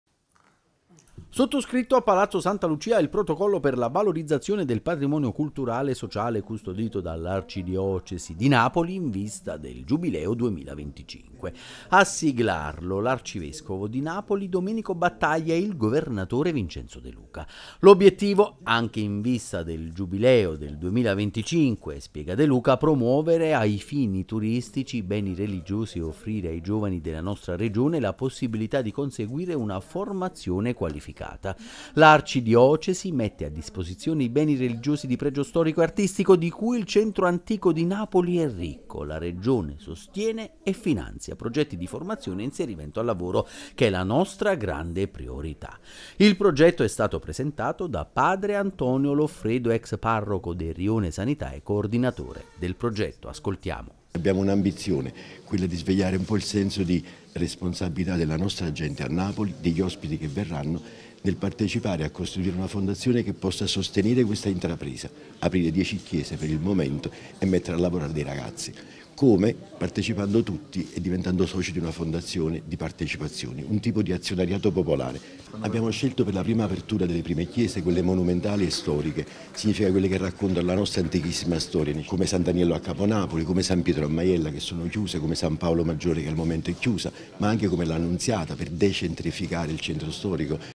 Giubileo 2025, intesa Curia-Regione: «Chiese riaperte e lavoro ai giovani»
Sottoscritto a Palazzo Santa Lucia il protocollo per la valorizzazione del patrimonio culturale e sociale custodito dall'arcidiocesi di Napoli in vista del Giubileo 2025. A siglarlo l'arcivescovo di Napoli Domenico Battaglia e il governatore Vincenzo De Luca. «L'obiettivo, anche in vista del Giubileo del 2025, è - spiega De Luca - promuovere a fini turistici i beni religiosi e offrire ai giovani della nostra regione la possibilità di conseguire una formazione qualificata. L'arcidiocesi mette a disposizione i beni religiosi di pregio storico e artistico di cui il centro antico di Napoli è ricco la Regione sostiene e finanzia progetti di formazione e inserimento al lavoro, che è la nostra grande priorità».

Il progetto è stato presentato da padre Antonio Loffredo, ex parroco del Rione Sanità e coordinatore del progetto: